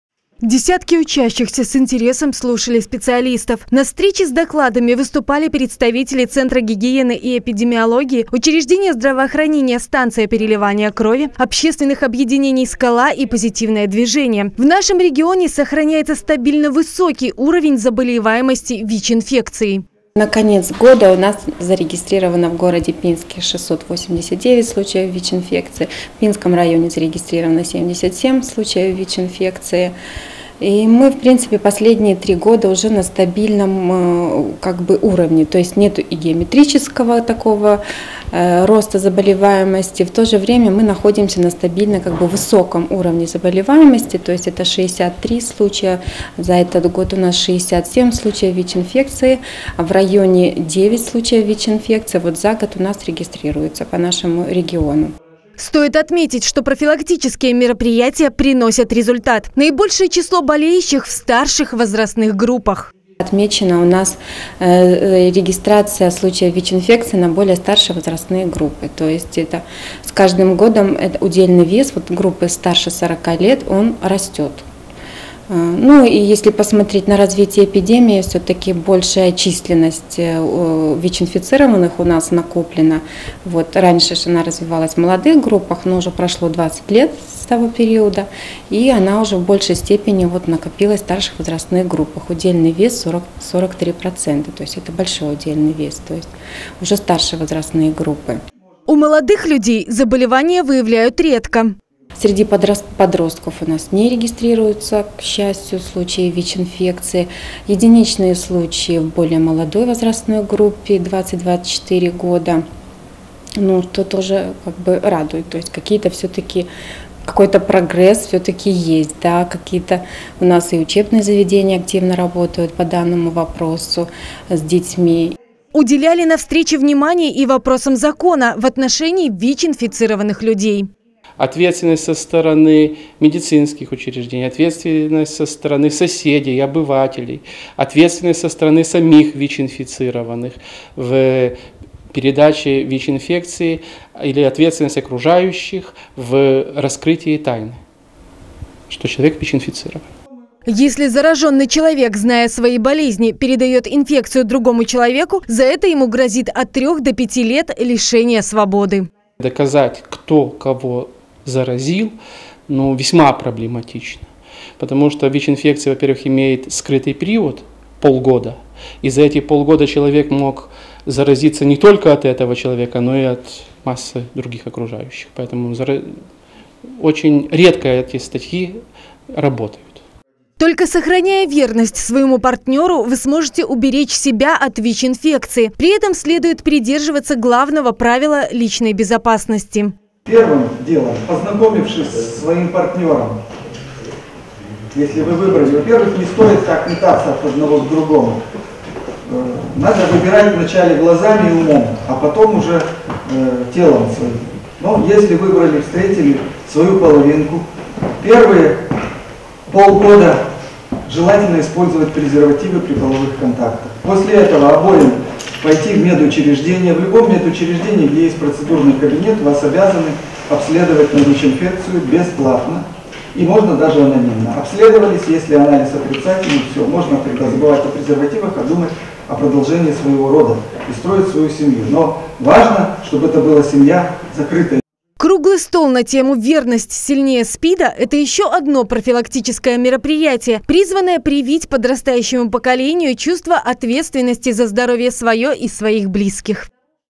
У молодых пинчан ВИЧ выявляют редко
Круглый стол на тему “Верность сильнее СПИДа”  провели  в территориальном центре социального обслуживания населения. На встречу пригласили учащихся средних учебных заведений Пинска. Наш город продолжает лидировать в Брестской области по количеству выявленных случаев ВИЧ и СПИД.